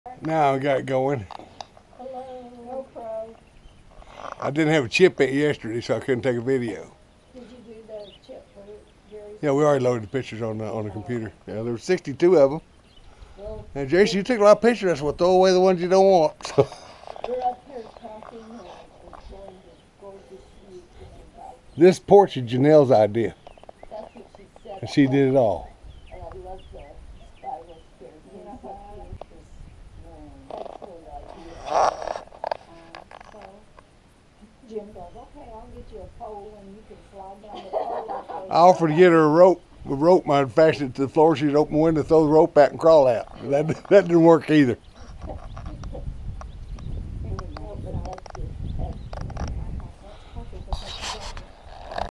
Janel and Judy on balcony
Judy Moody and Janel on our balcony. Great visit and new friend!